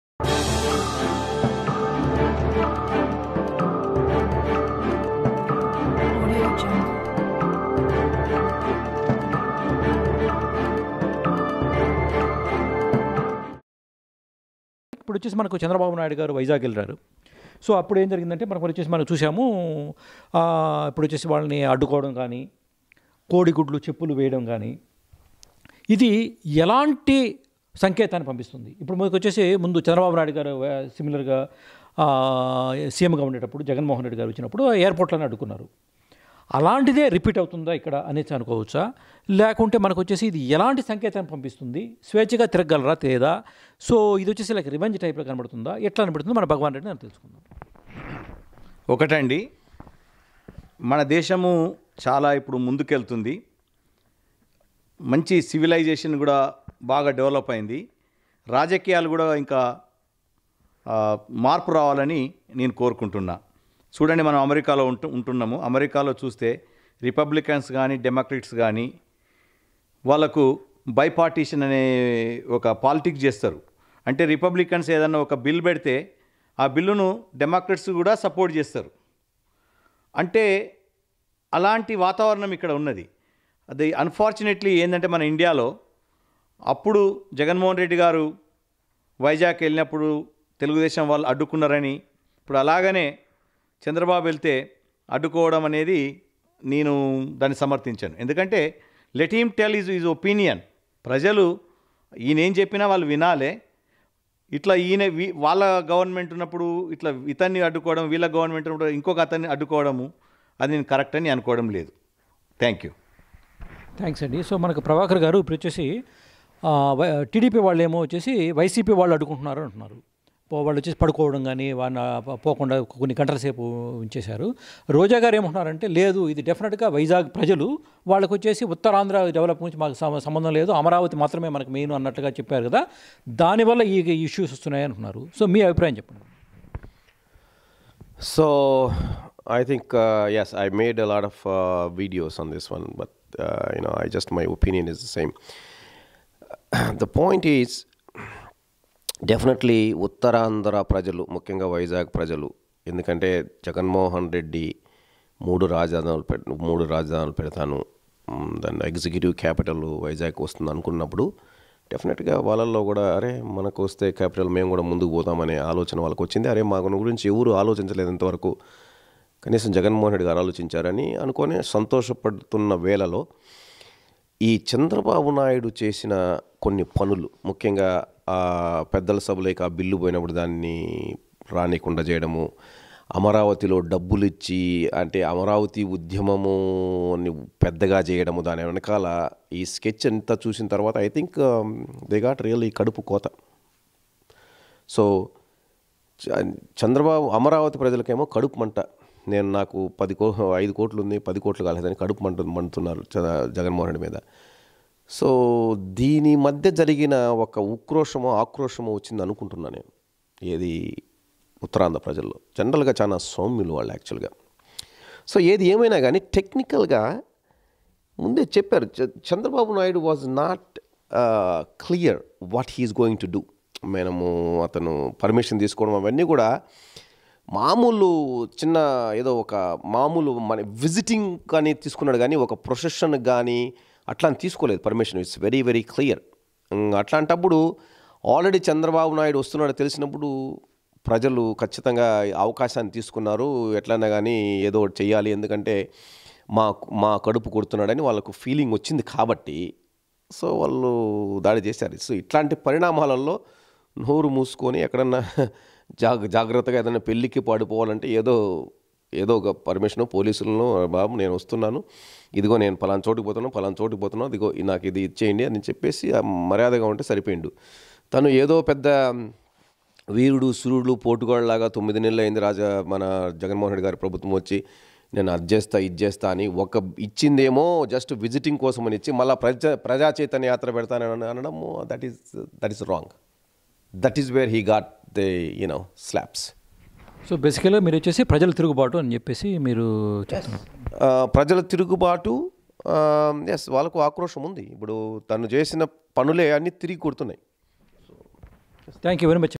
Chandrababu Naidu Vizag Tour Controversy: Political Heat In AP with GKTV
Chandrababu Naidu Vizag Tour Controversy: Political Heat In AP with Punch Prabhakar and friends  | TDP VS YCP | GKTVusa